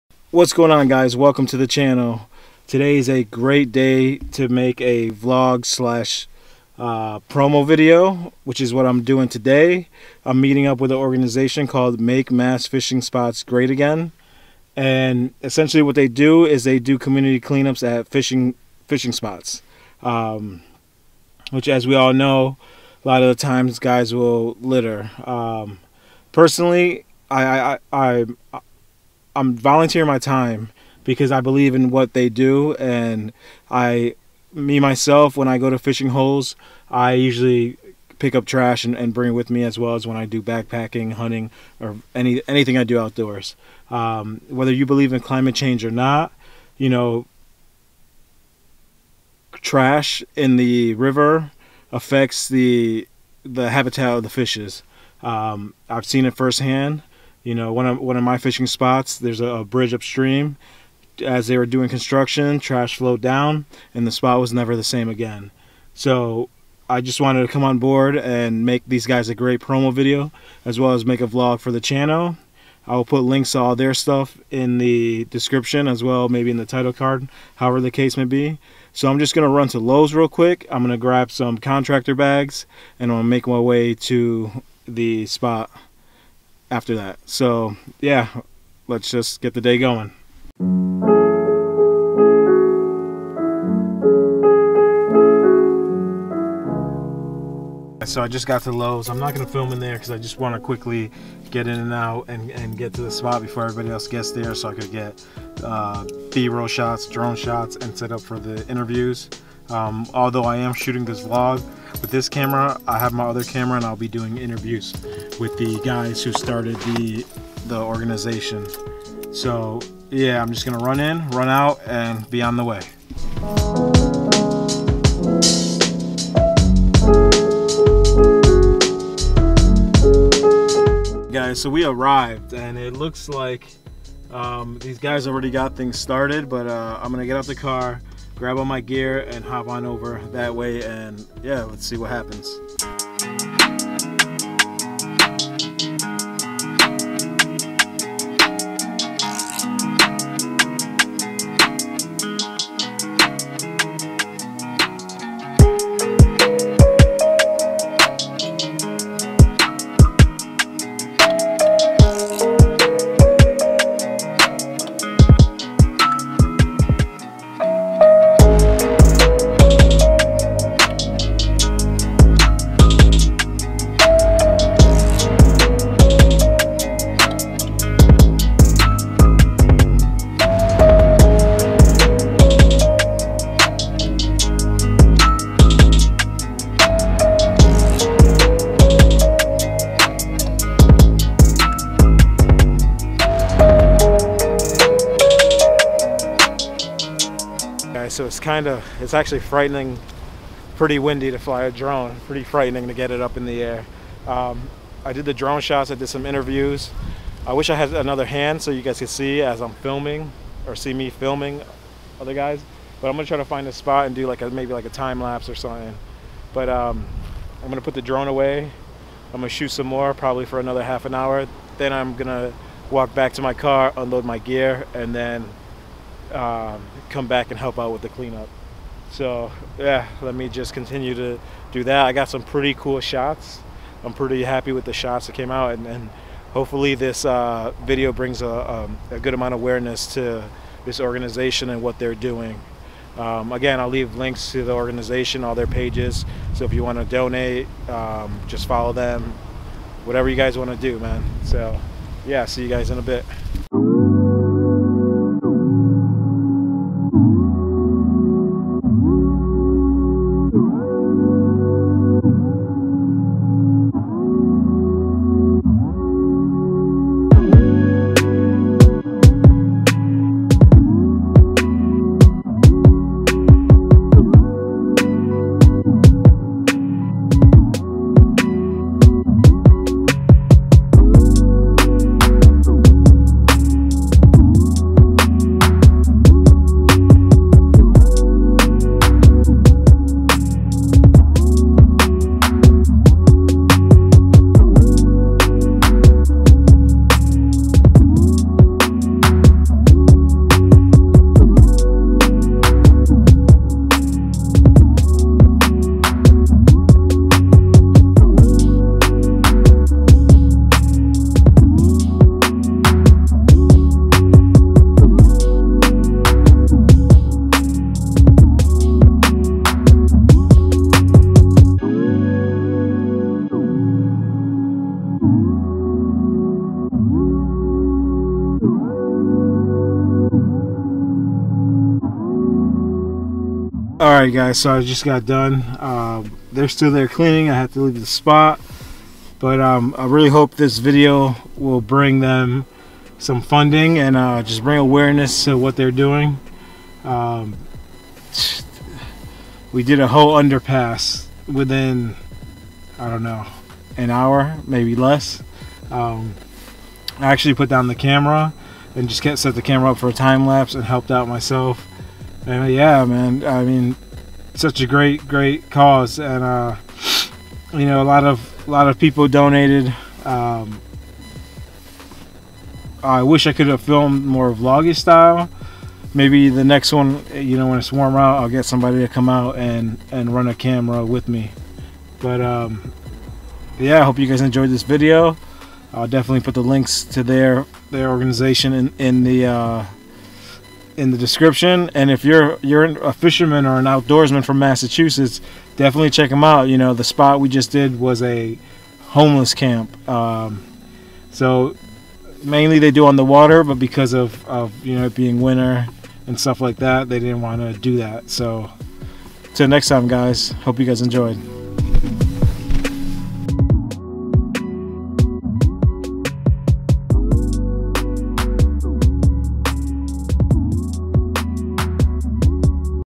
MMFSGA - I391 CLEANUP Holyoke Ma
I join Make MA Fishing Spots Great Again (mmfsga) in a homeless camp clean up. Shooting a promo for the organization.
Keep Up with them and find out what they are up